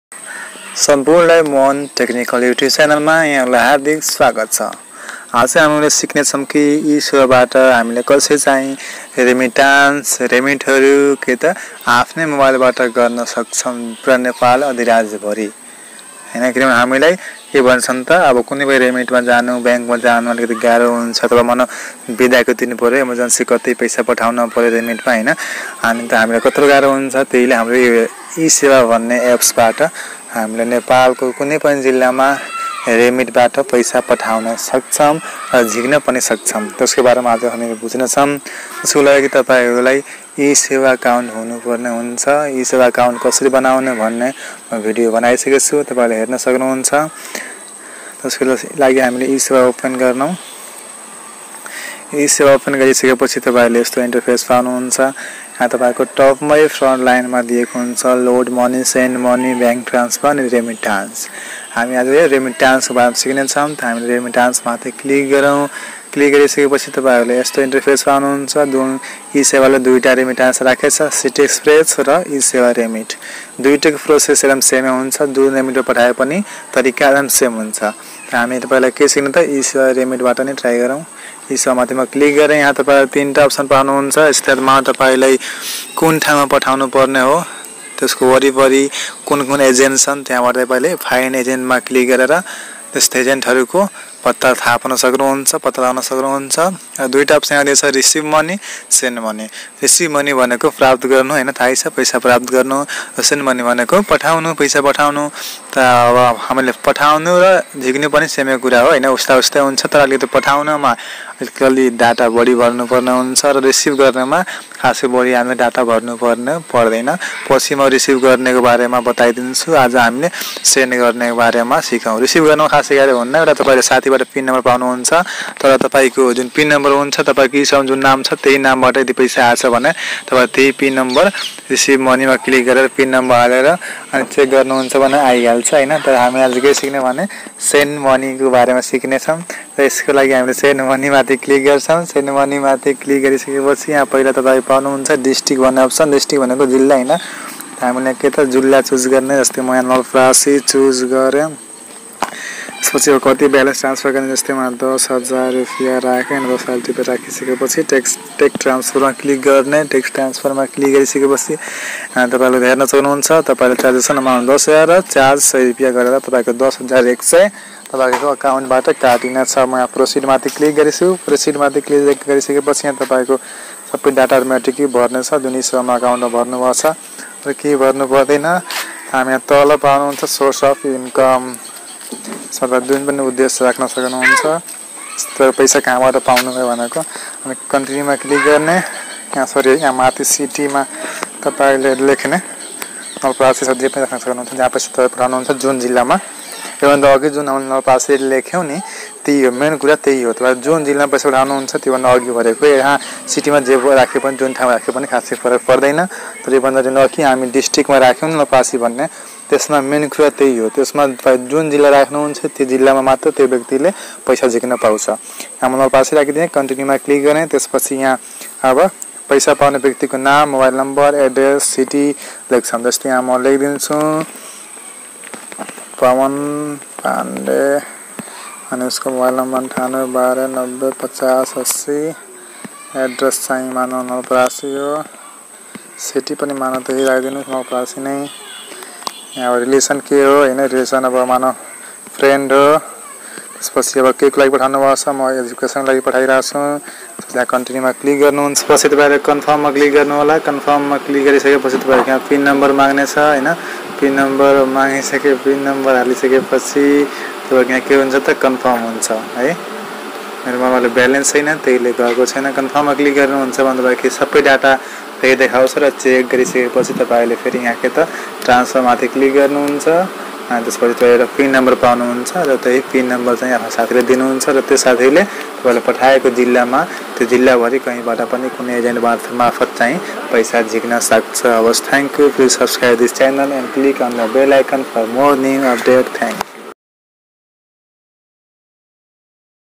How to send domestic remit money transfer from eSewa Dogital mobile ,Sewa Remit, City Express Remit
Today i show in this video to how to send domestic remit money  transfer from eSewa Digital Mobile wallet to eSewa Remit and City Express Remit.

Please Like l Share l Comment your sweet word for help our channel . we are always solve your problem on time.

आज हामी यश भिडियो मा नेपाल अधिराज्य भरि रकम कसरी पठाउन सकिन्छ भन्ने बारे सिक्ने छौ ।

केही शिर्षक मा भिडियो चाहिएमा कमेन्ट गर्न सक्नु हुन्छ । तपाईंका हरेक समस्याको समाधानका उपाय छिटोभन्दा छिटो गर्न प्रयास सधैं रहने छ ।

City Express Remit
eSewa Money Transfer
Domeatic Remit